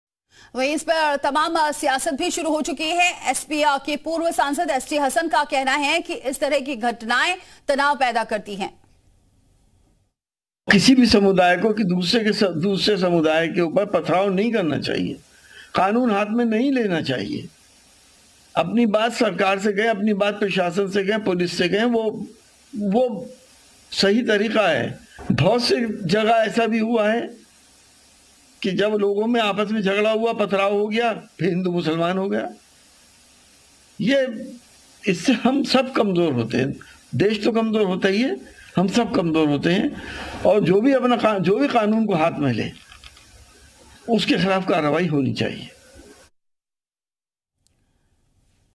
Bahraich Murti Visarjan Violence Update: बहराइच में पर क्या बोले SP नेता ST Hassan? | Breaking News
Bahraich: बहराइच में दुर्गा मूर्ति विसर्जन के दौरान हुआ बवाल, सपा नेता एसटी हसन का बयान, 'दोनों समुदाय को ऐसा नहीं करना चाहिए', 'सरकार को सख्त कार्रवाई करने की जरूरत'

Bahraich: Ruckus during Durga idol immersion in Bahraich, SP leader ST Hasan's statement, 'Both communities should not do this', 'Government needs to take strict action'